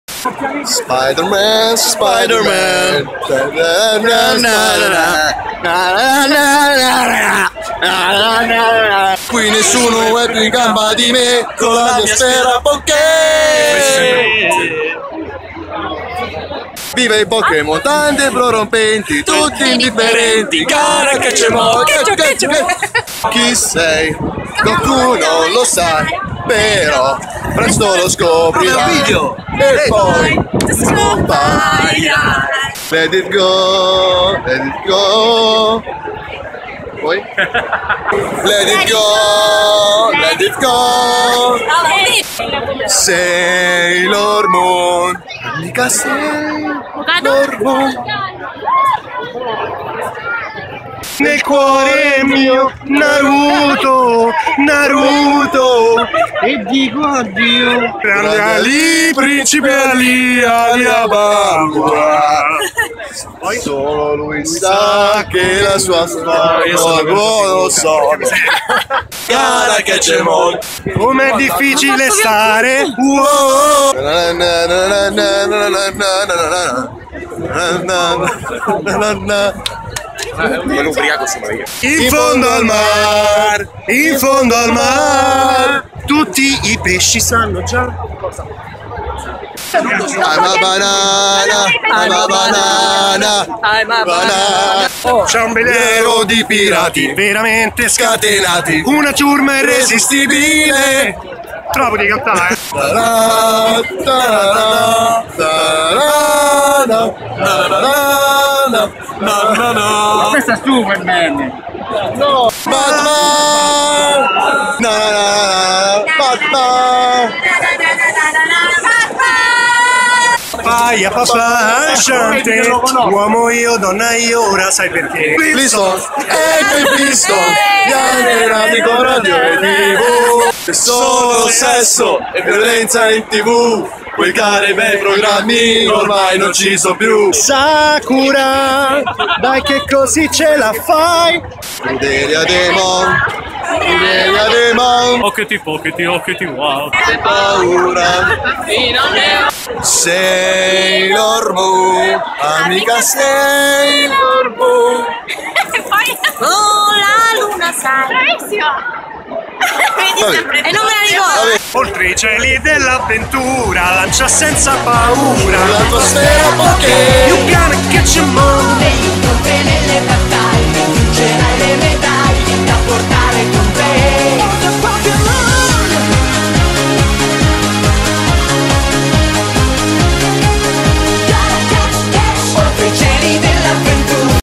Cantare le SIGLE con i personaggi dei CARTONI ANIMATI
Oggi sono andato in giro a cantare le SIGLE più famose insieme ai Personaggi dei CARTONI ANIMATI
Arriviamo a 500 Like? Ci vediamo la prossima settimana.
Grazie a Cinzix, MondoPaolo e il Gigante sul Tubo.